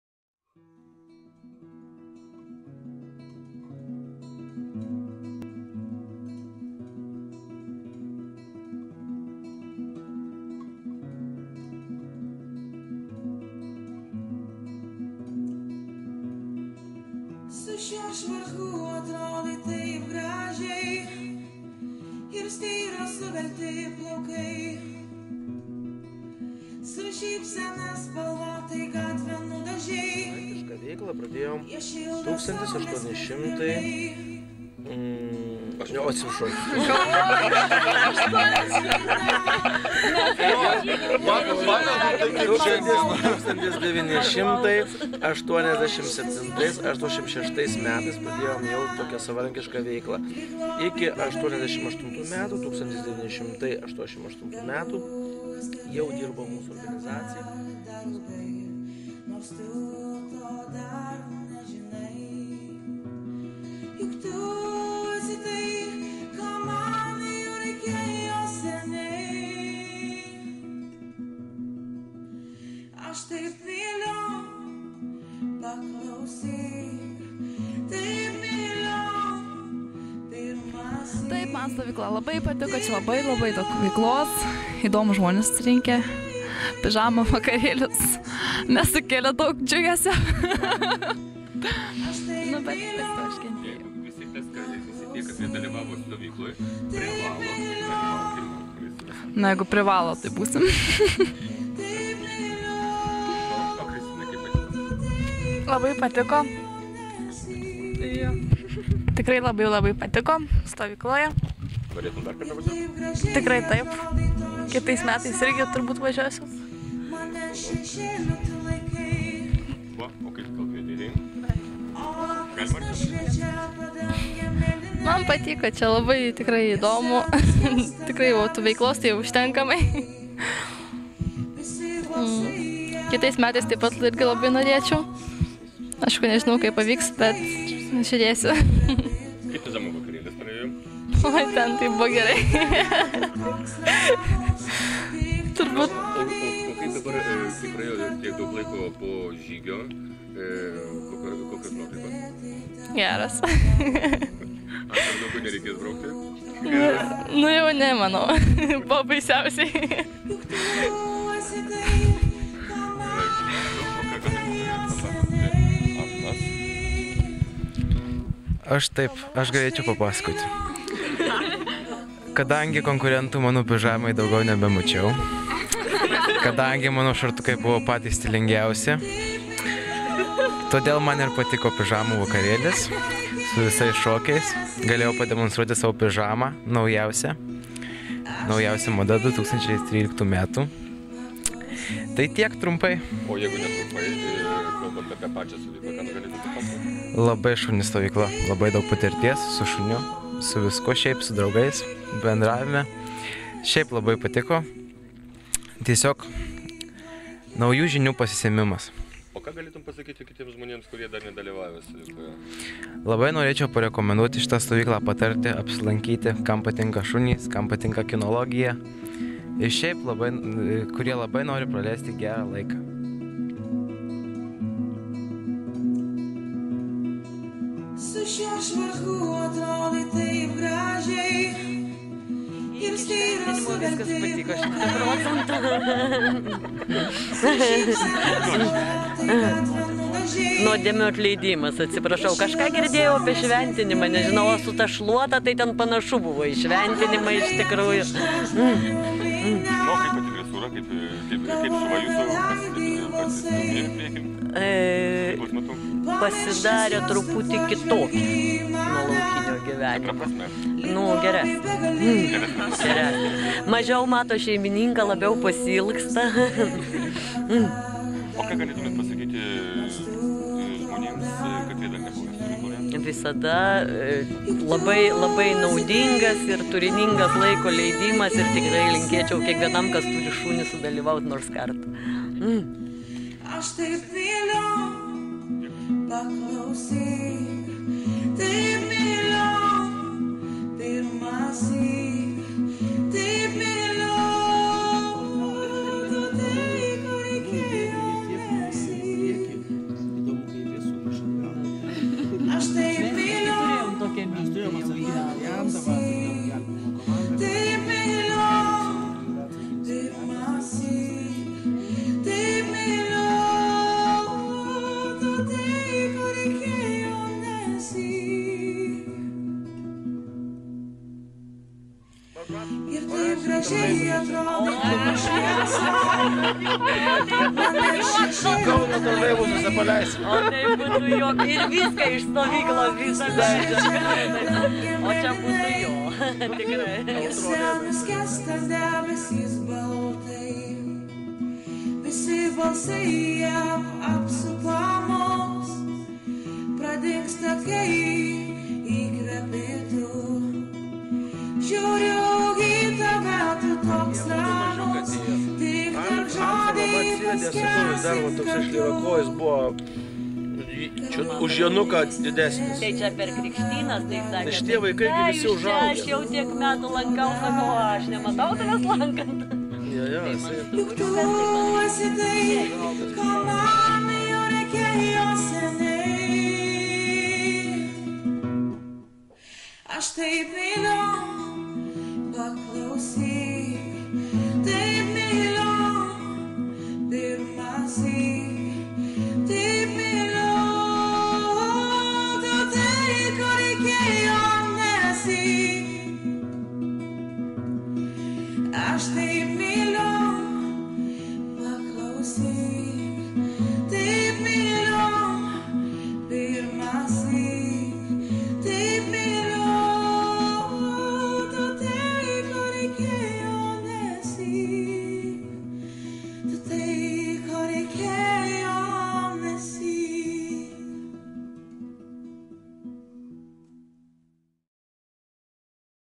Nuaro Kinologijos stovykla 2013 PRIE ŽVAKIŲ ŠVIESOS
Paskutinė diena 2013m. stovykloje. Dalyvių refleksijos žvakių šviesoje. Meilės muzikos galite paklausyti jos kanale Meilė- aš taip myliu